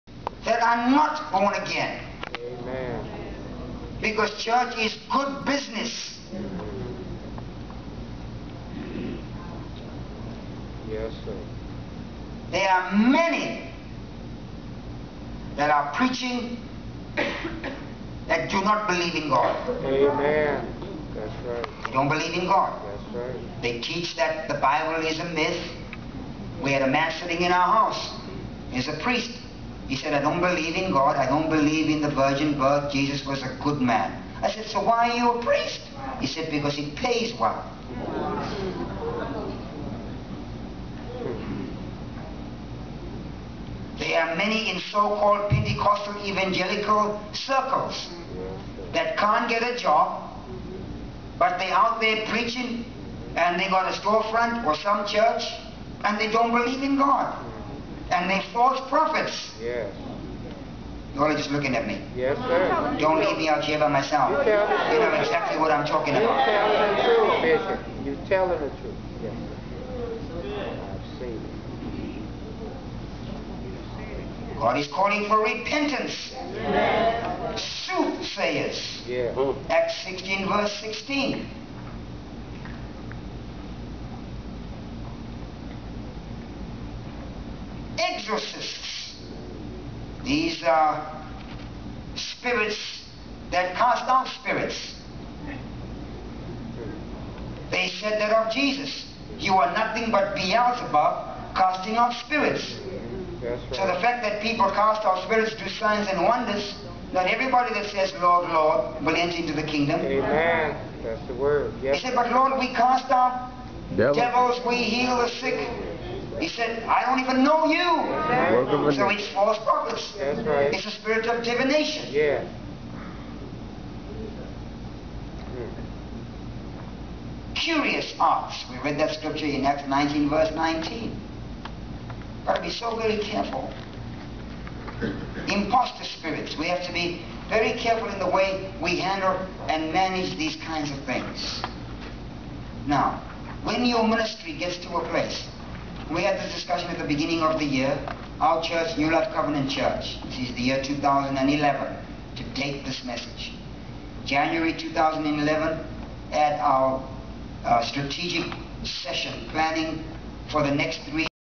7-22-11 HGIM Regional Summit Speaker: Apostle Tudor Bismark
Clip 10
Spirit Mediums, Many Preach & Do not Believe in God.